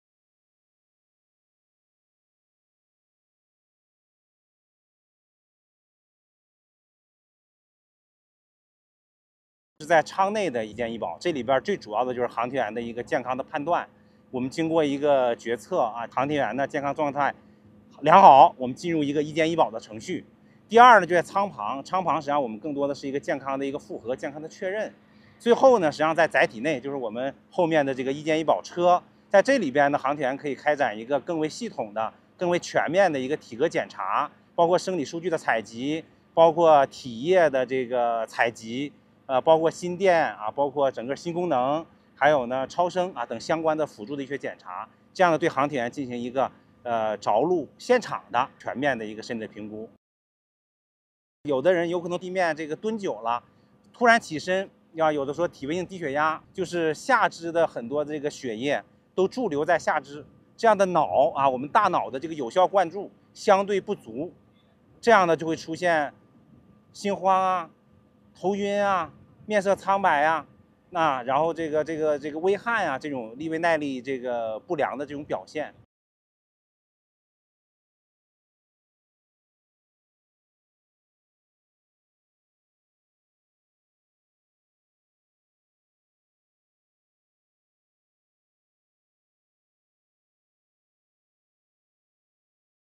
Shenzhou-17 Crew All in Good Health Back on Earth
The three astronauts of the Shenzhou-17 mission, after staying in orbit for 187 days, are all in good health condition, and will enter a period of medical quarantine, according to medics in the Shenzhou-17 search and retrieval team.